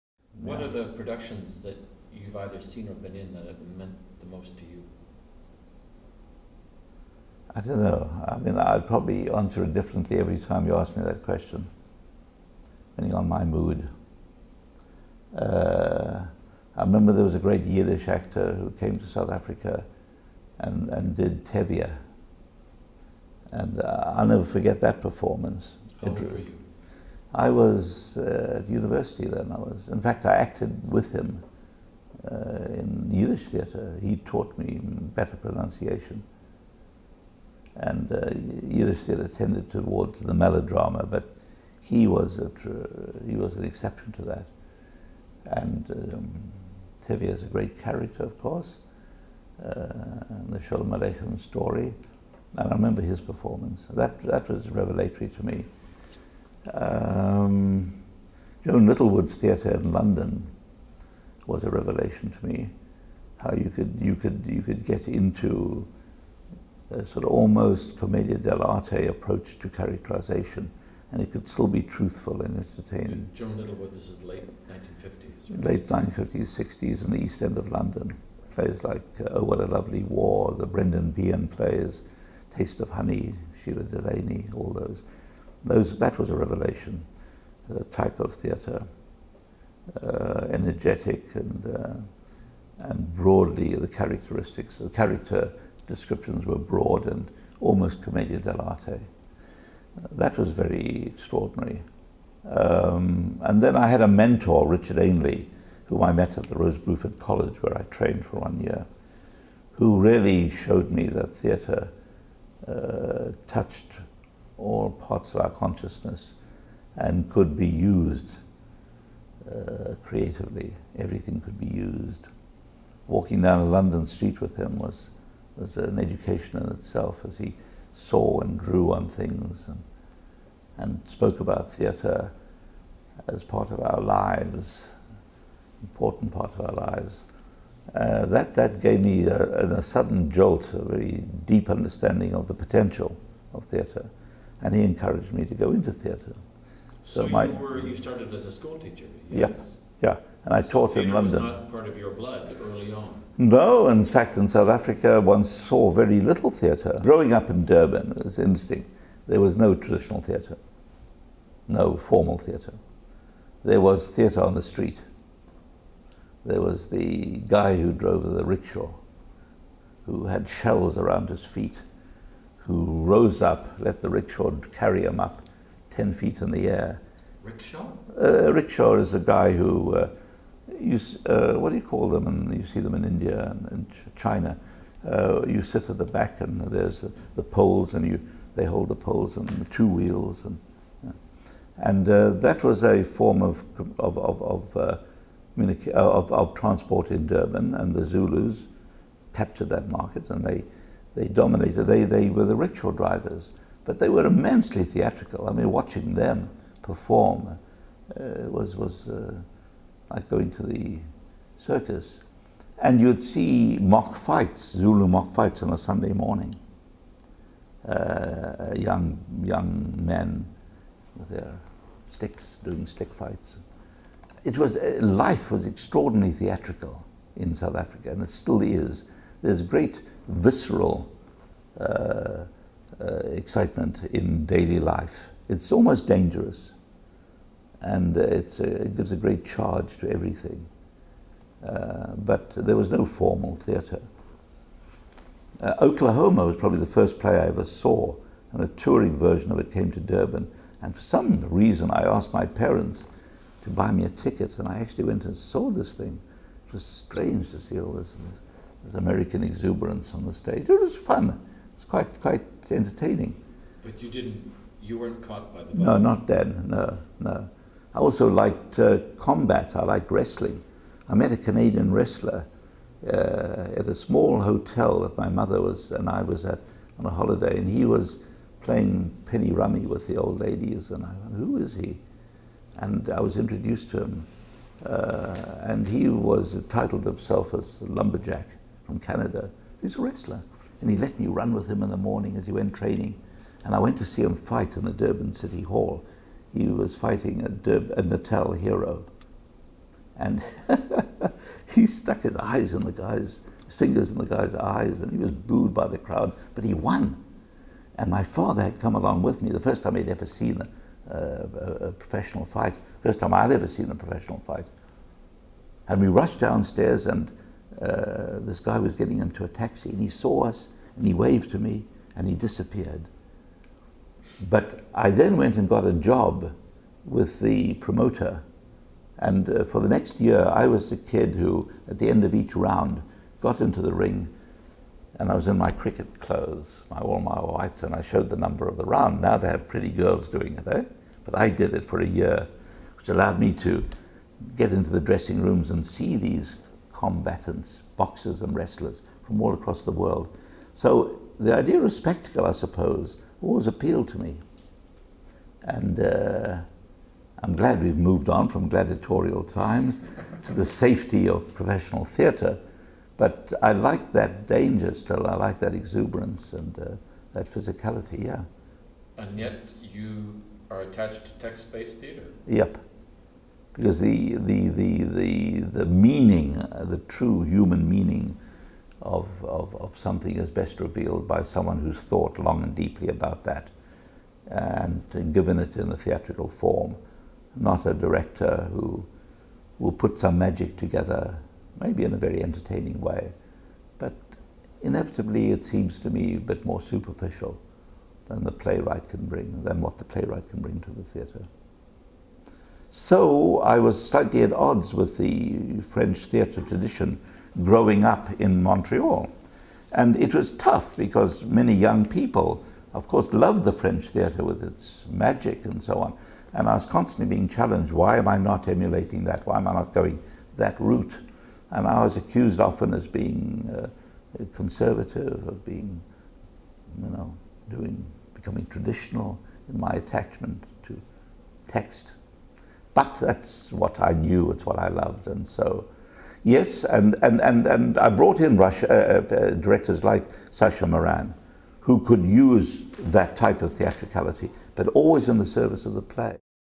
Maurice Podbrey on starting out and memorable productions (Part 6 of 9)
Maurice Podbrey on starting out and memorable productions.

This interview was conducted by R.H. Thomson on May 30, 2007. Filming location courtesy of Hart House Theatre.